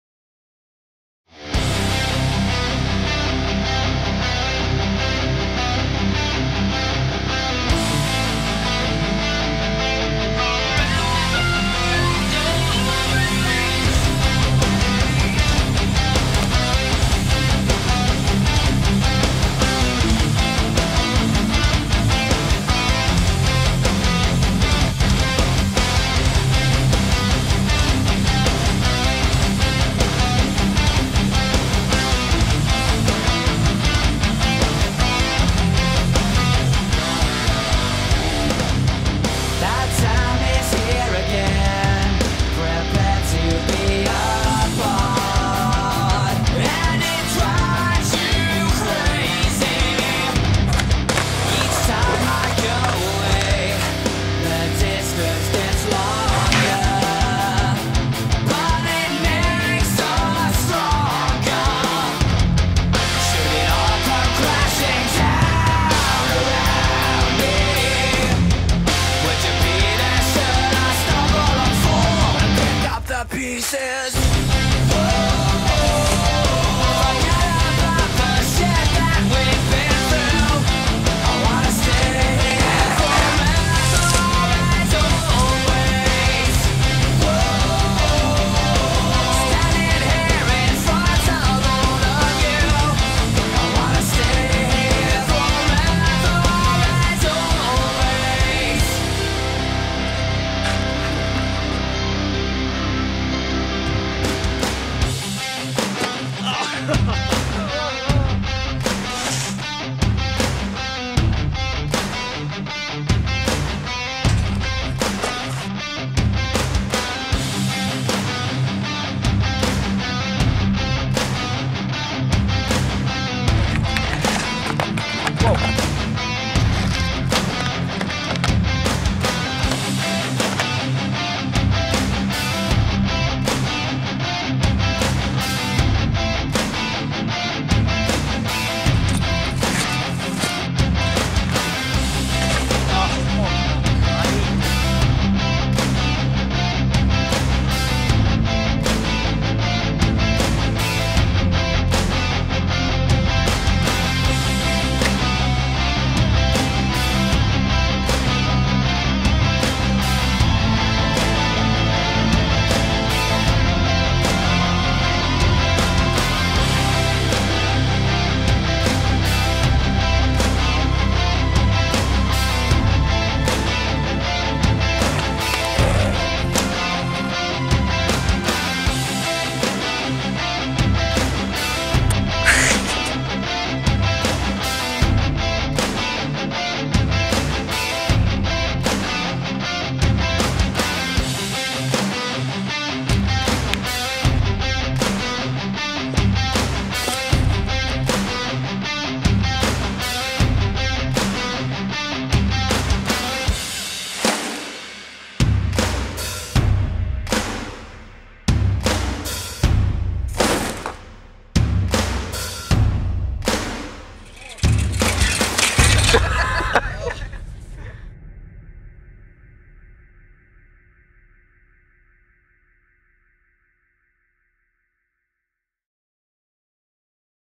sOmmer 2o1O
Freerunning, parkour, diving, skateboarding... and just having fuuuuunnnnnnnn !